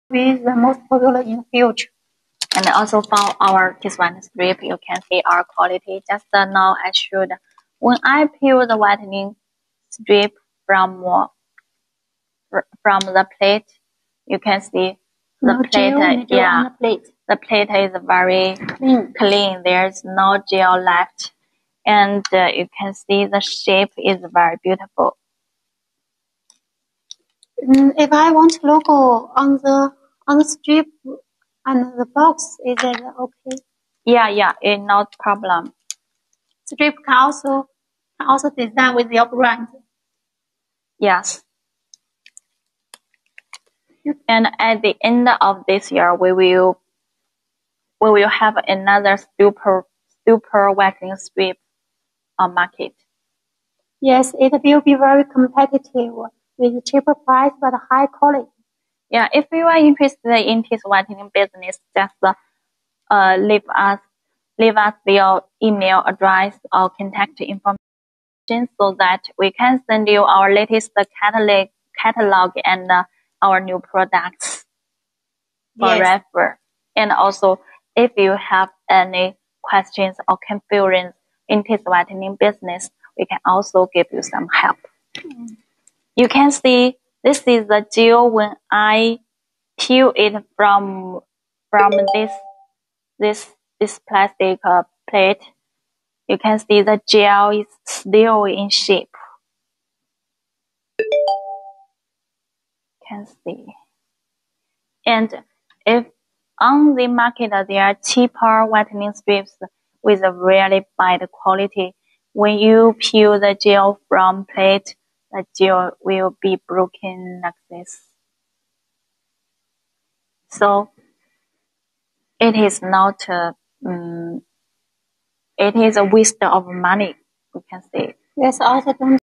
Teeth Whitening Strips Quality Evaluation
Teeth whitening strips private label give you a comfortable at home teeth whitening experience, uses the same enamel-safe teeth whitening ingredients dentists use, improvement 4-8 shade guide finally.

Our Teeth Whitening Strips private label OEM is avalible and will make you satisified.